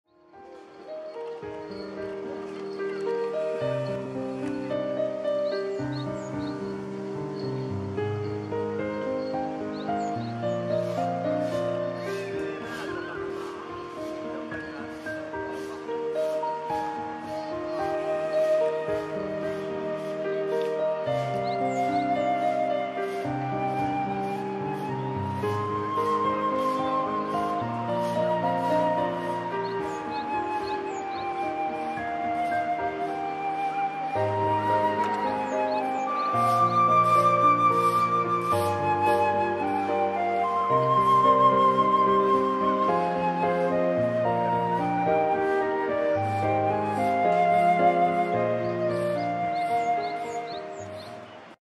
カイツブリの幼鳥 餌取り
親元を離れたカイツブリの幼鳥が水面に浮く虫などを食べています。まだ顔にゼブラ模様が残っています。